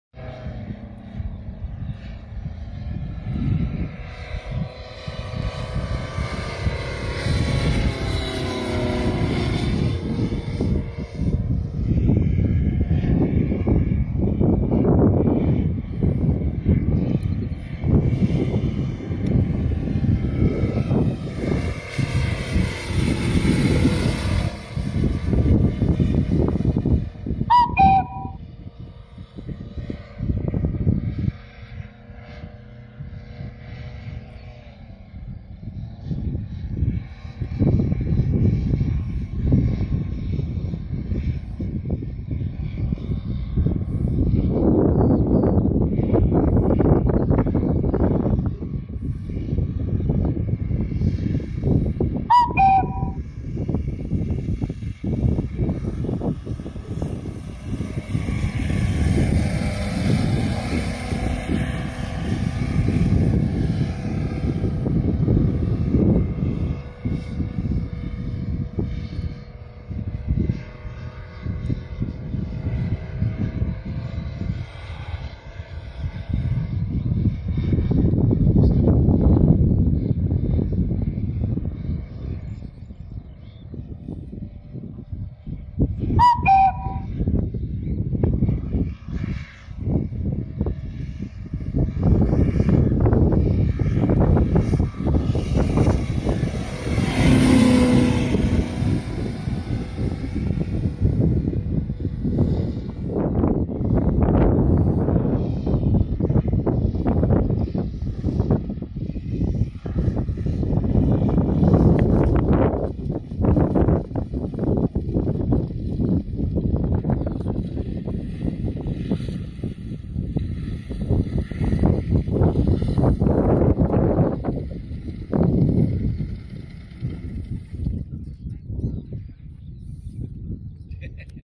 My Fokker D-VII himmelfahrt 2022 Bad Langensalza
Short movie made with iPhone during my visit to Modellflugclub Bad Langensalza May 2022. This was the 9th flight of the week. Wind speed this flight 3 à 4 bft, no problem for the model.